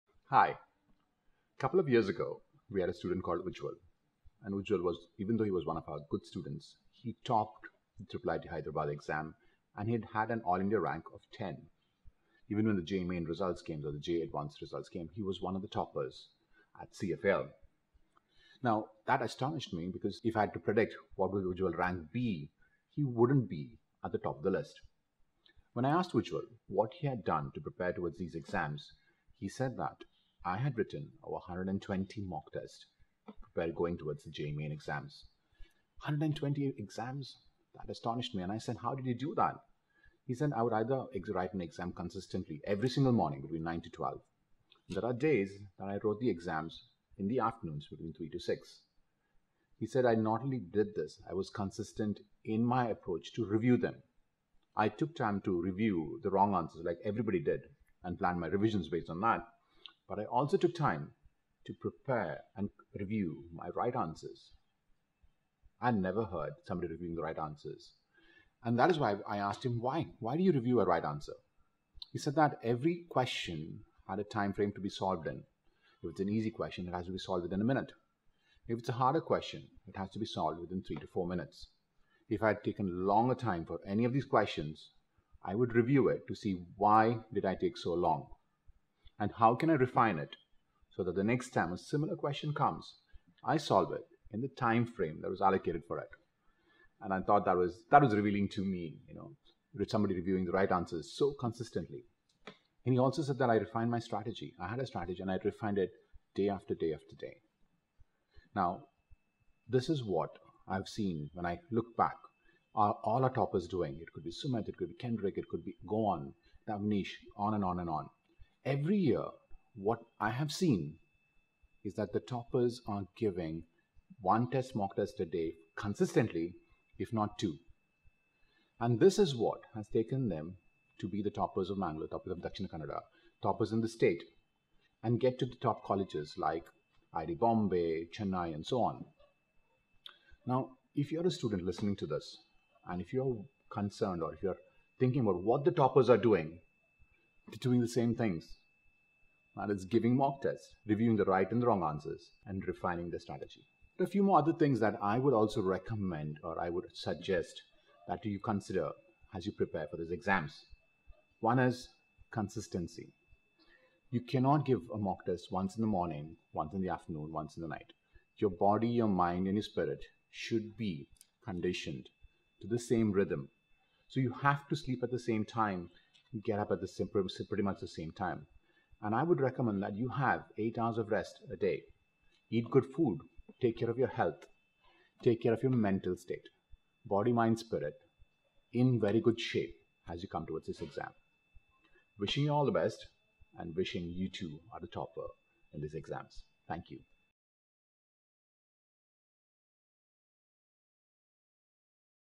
How many mock tests should you give for JEE Main 2022 ? Toppers' Secret | CFAL Mangalore
As students prepare for any competitive exams, many ask questions or make mistakes. Most students are unable to decide whether to revise or to solve mock papers. Watch our Program Coordinator, Vijay Moras, share a story from one of our students, who prepared and topped the UGEE & JEE main exams.
for info on preparing for JEE / NEET
Follow us on the Top Social media Websites: